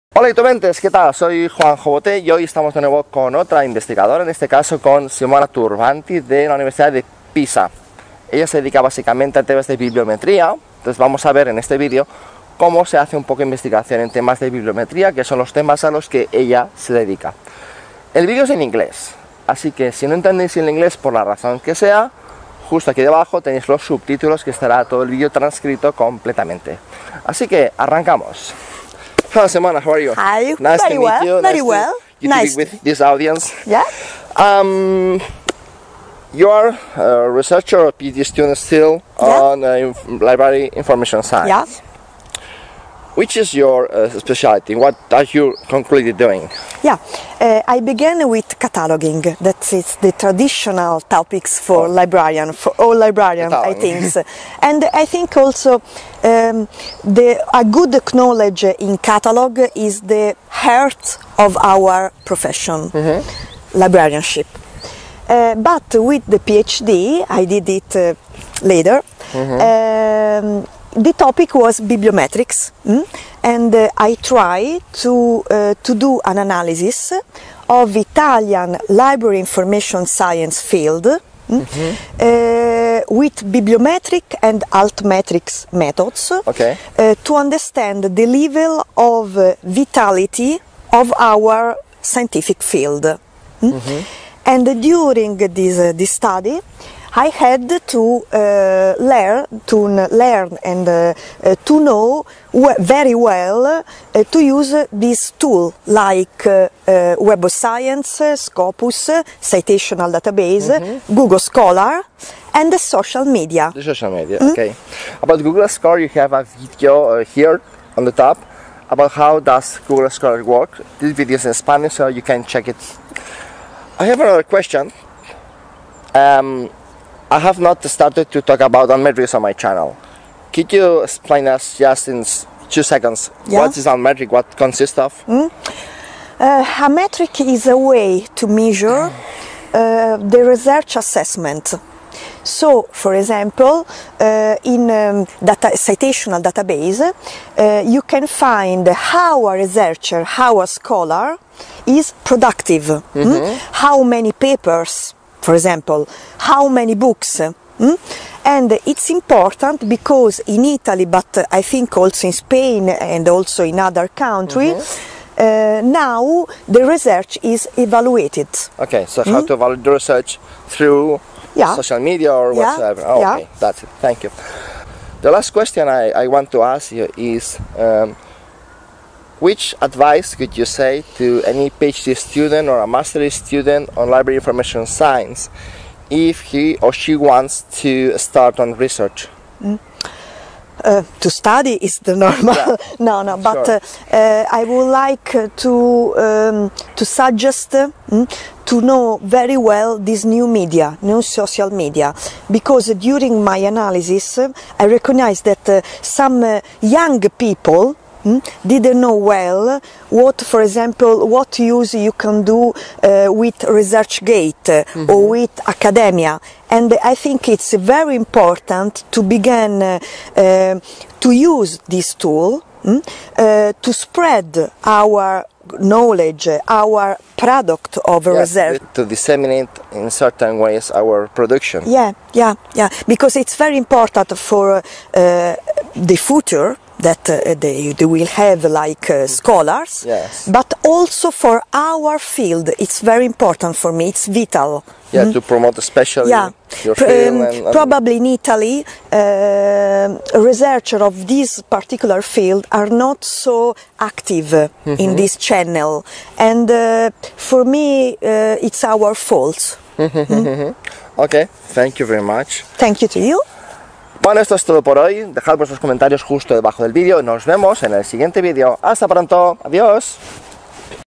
Qué es la bibliometría - Entrevista con Simona Turbanti - Bibliotecas y Ciencias de la Información
En este video en una entrevista con Simona Turbanti de la Universidad de Pisa puedes ver que es la Bibliometria y cómo se investiga.

Juanjo Boté es formador.

twitter: @juanjobote enviar mensaje directo
(enviar mensaje a la página)

Días de emisión
Miércoles a las 20.00 pm